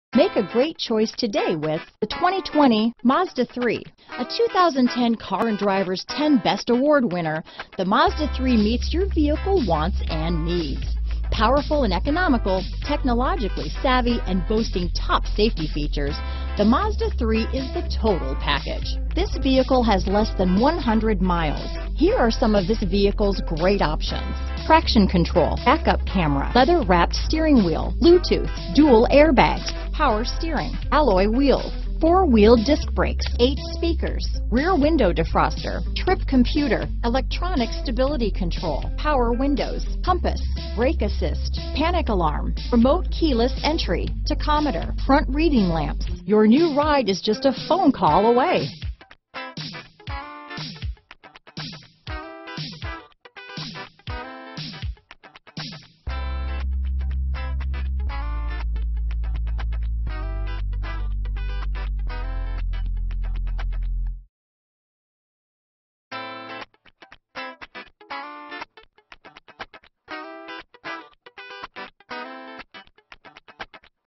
2020 Mazda Mazda3 at Oxmoor Mazda | Louisville & Lexington, KY M14955
Sonic Silver Metallic New 2020 Mazda Mazda3 available at Oxmoor Mazda in Louisville, Kentucky. Servicing the Lexington, Elizabethtown, KY New Albany, IN Jeffersonville, IN area.

Oxmoor Mazda
7913 Shelbyville Rd

2020 Mazda Mazda3 Sonic Silver Metallic **APPLE CARPLAY**, ALLOY WHEELS, Blind Spot Monitoring w/Rear Cross Traffic Alert, BLIND-SPOT MONITOR, BLUETOOTH TECHNOLOGY, Dual Zone Automatic Climate Control, Leather Steering Wheel & Shift Knob, Mazda Advanced Keyless Entry, MAZDA CONNECT, PUSH BUTTON START, Rear Armrest w/Cup Holders, REAR BACKUP CAMERA, Select Package, Side Mirror Turn Lamps, TOUCH SCREEN DISPLAY. 26/35 City/Highway MPG  $1000 - Customer Cash. Exp. 08/31/2020